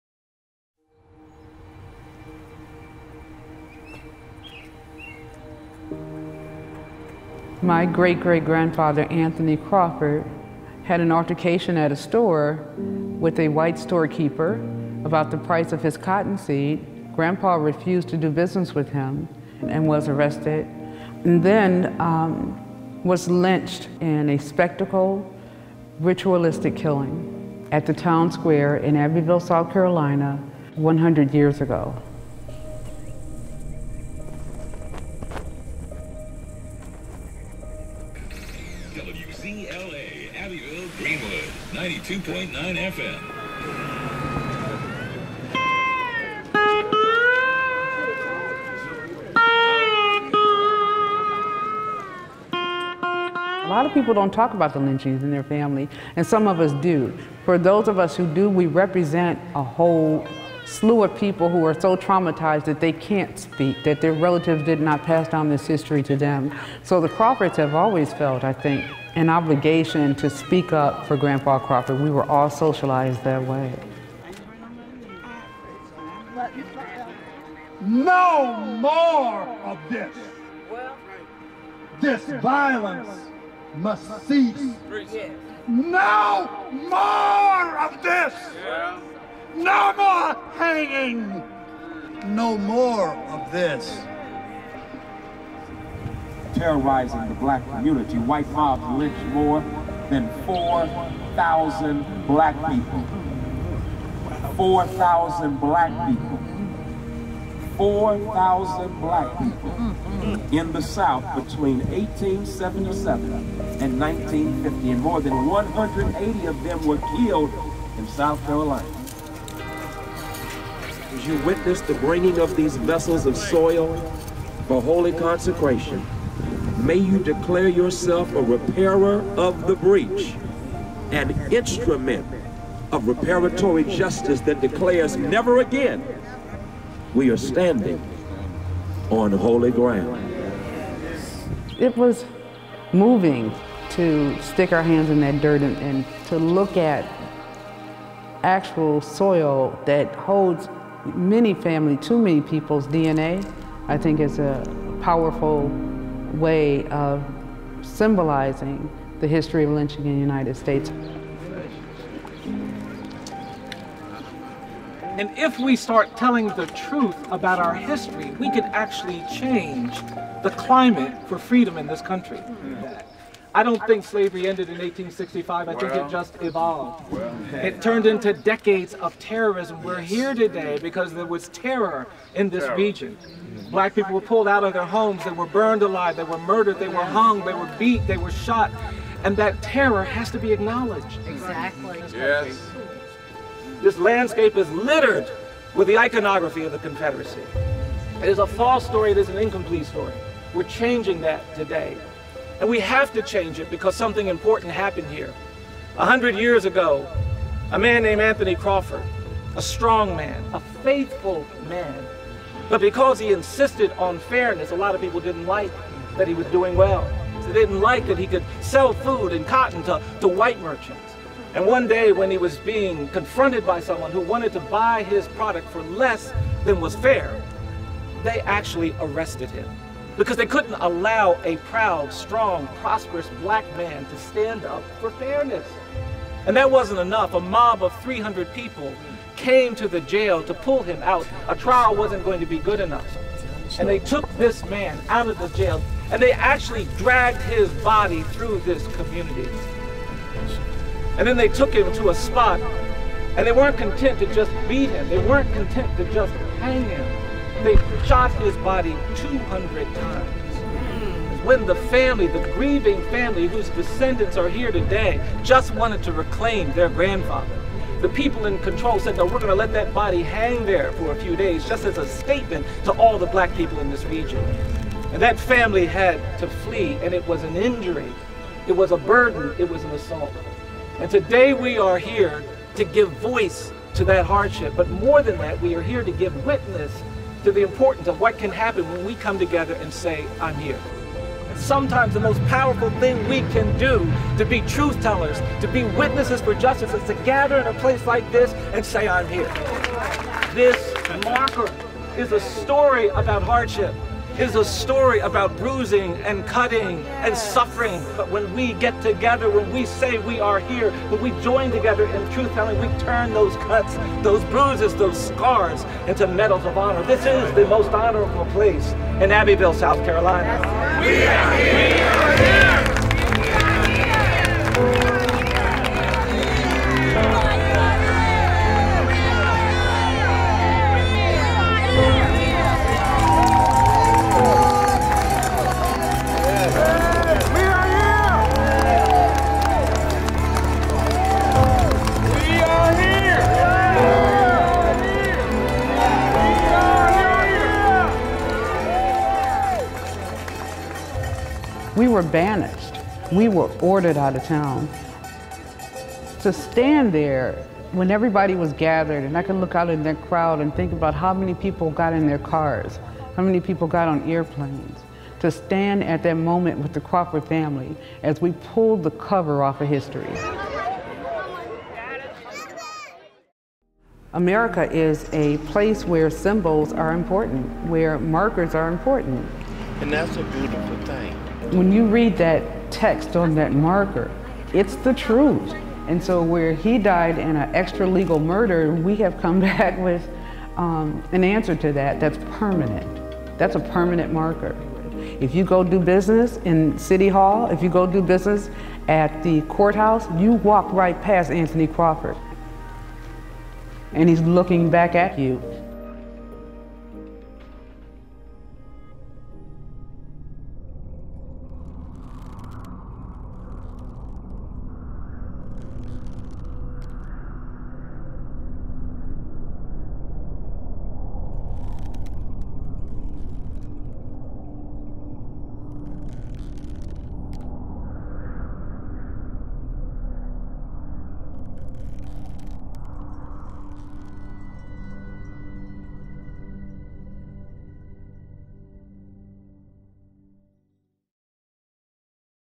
Abbeville
A community's response to racial terror lynching.

The Equal Justice Initiative works to end mass incarceration, excessive punishment, and racial inequality.